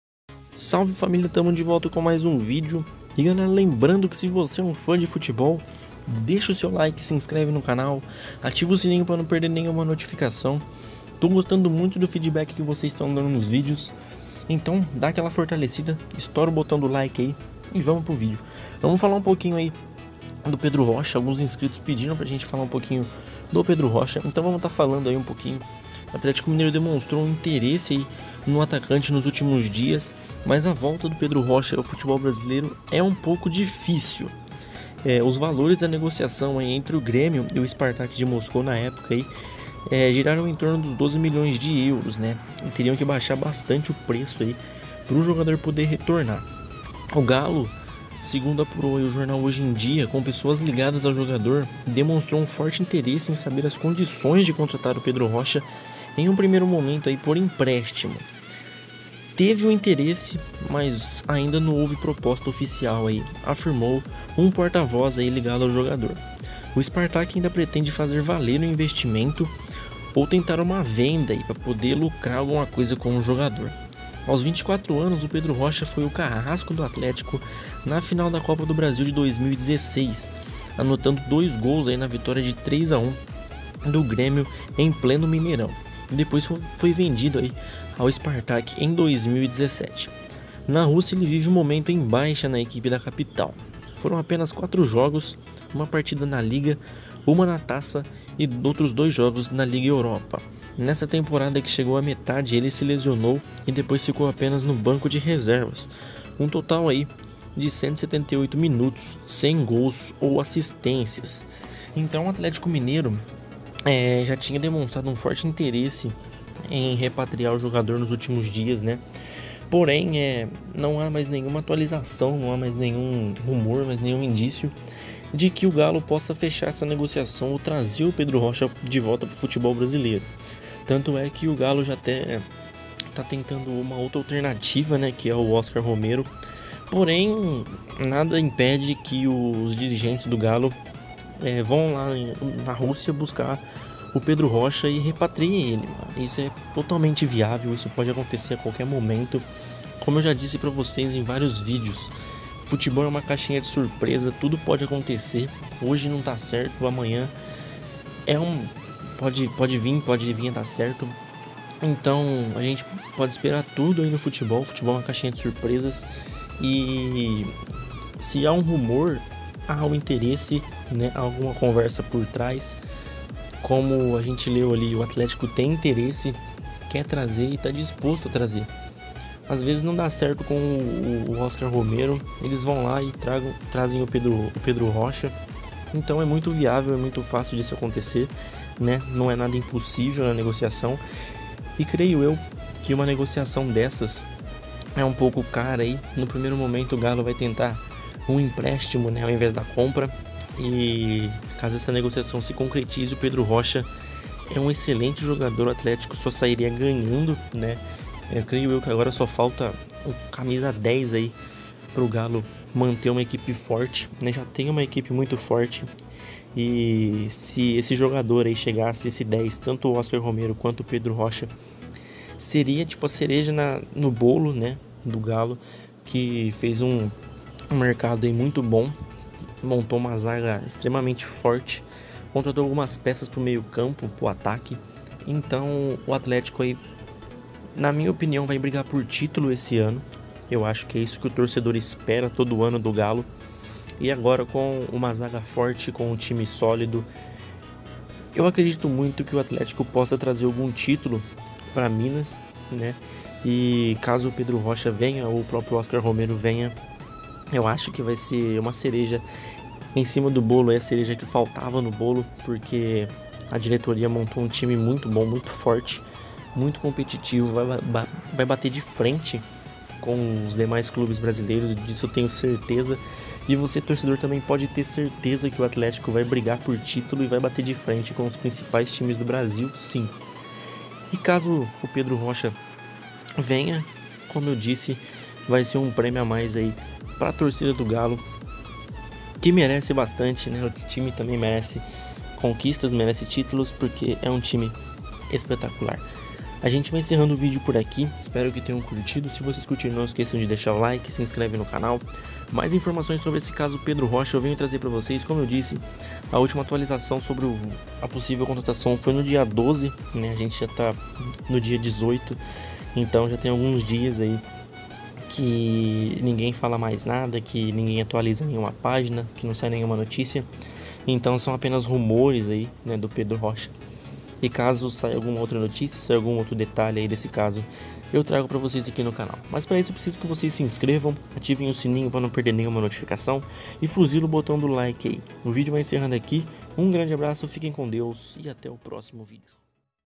Notícias do Atlético Mineiro: Pedro Rocha vem pro Atlético ou não ? Confira !
A pedido de alguns inscritos vamos falar um pouco sobre Pedro Rocha e Atlético Mineiro, o Galo tem forte interesse no jogador porém a contratação junto ao Spartack é difícil.